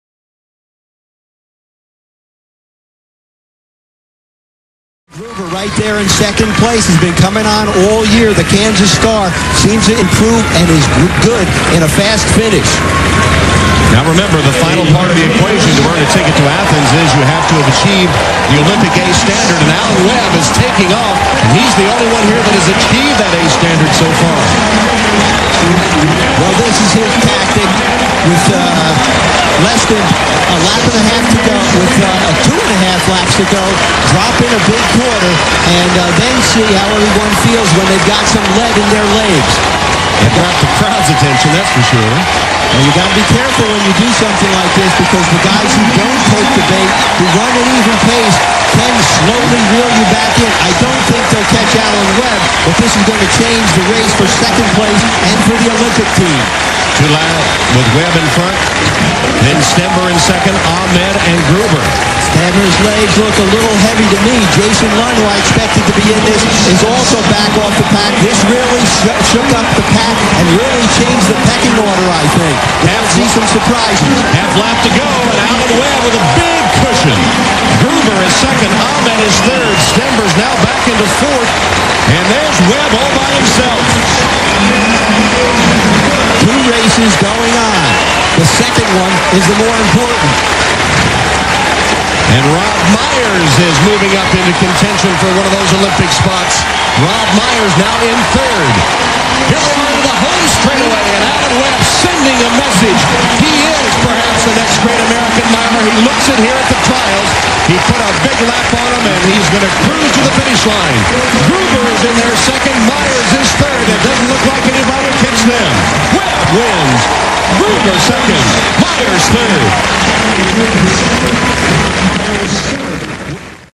2004 1500m Olympic Trial
Alan Webb dominates the 2004 trials.